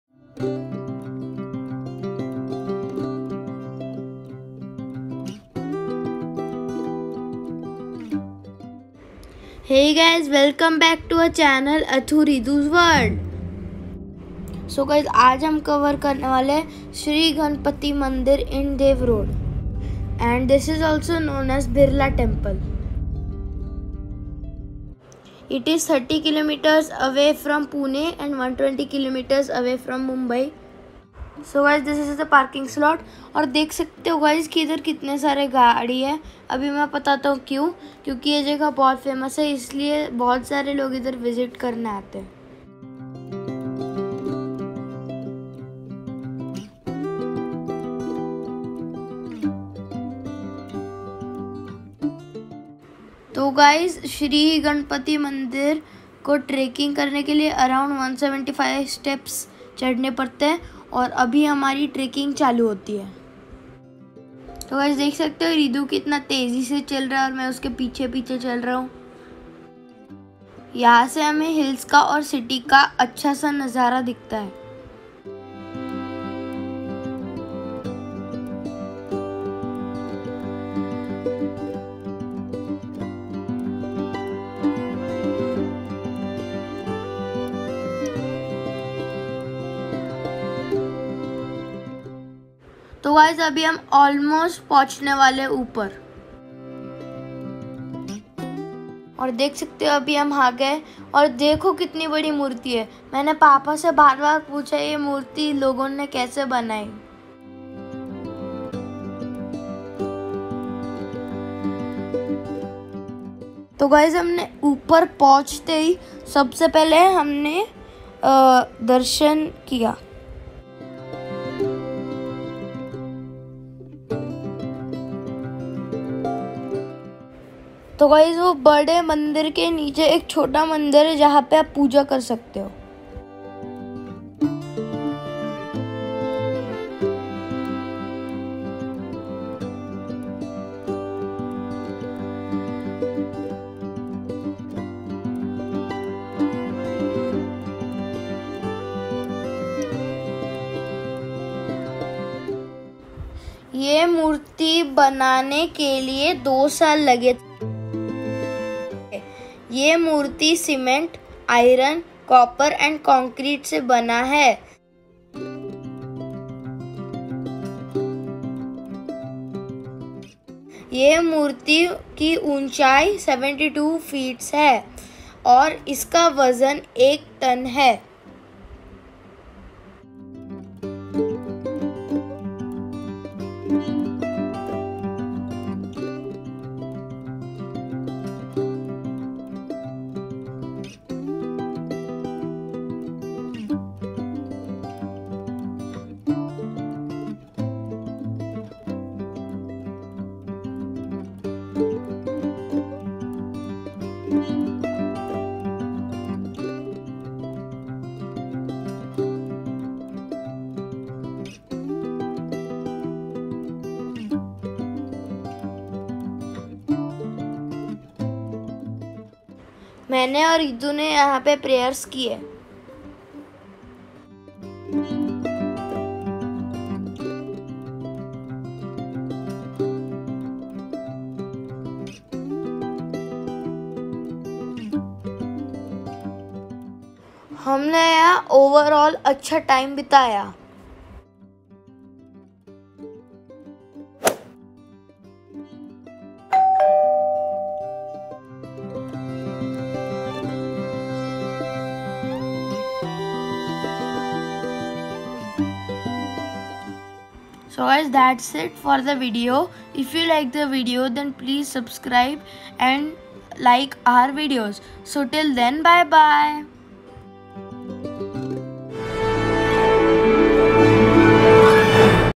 Dehu Road | Birla Ganpati Temple | weekend getaways Pune Mumbai | Nearby | Travel Vlog Maharashtra
Birla Ganpati Temple
The Shree Ganpati Bappa Temple is situated on Dehu Road, it is near to Dehu toll Naka.

It is on the old Pune Mumbai highway.
It is 30 km from Pune & 120 km from Mumbai.
This murti is made up of Iron Concrete Copper & Cement. Height of this murti is 72 ft & weight is almost 1 Ton. We need to take around 175 steps to reach on the top.

This is one of the weekend attractions or frequently visited place by Pune & Mumbai people. you can visit the temple with family & friends.

Entry is free however we need to pay for parkings.
Parking area is available for Four & two wheelers.